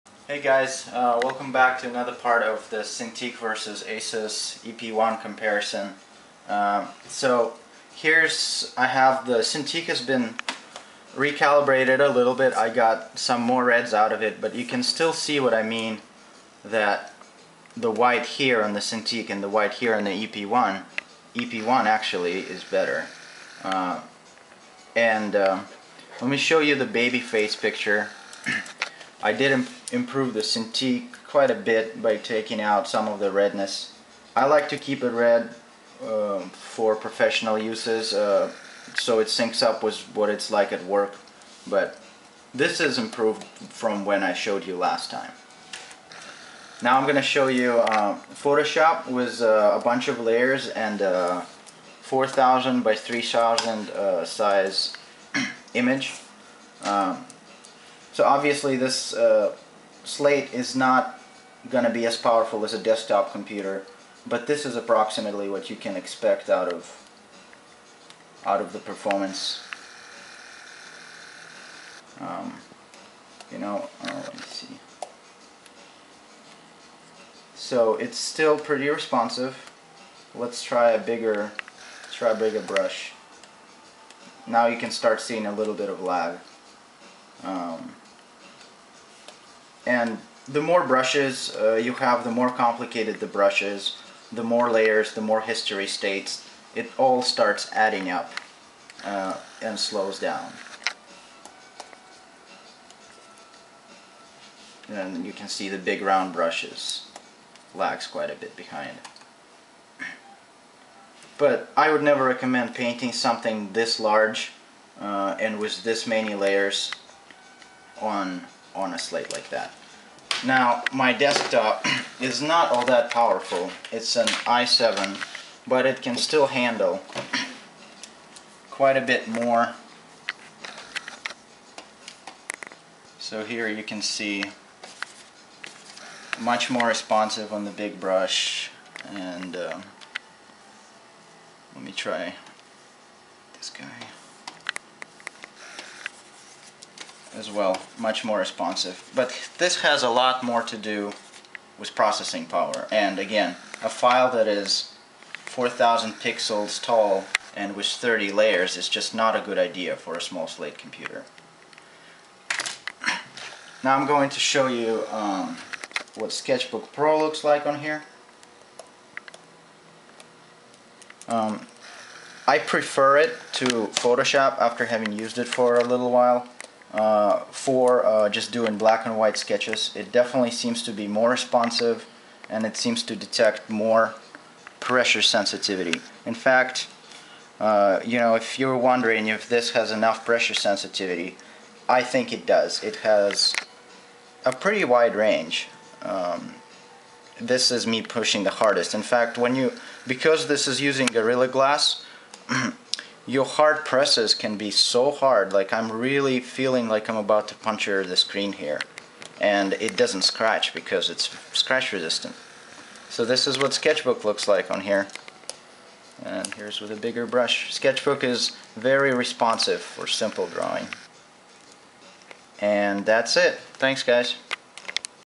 ASUS EP121 vs Cintiq 21UX Part 2
In this video I show you the Photoshop perfomance of larger files (over 100mb), Sketchbook Pro on the slate, and further screen comparisons.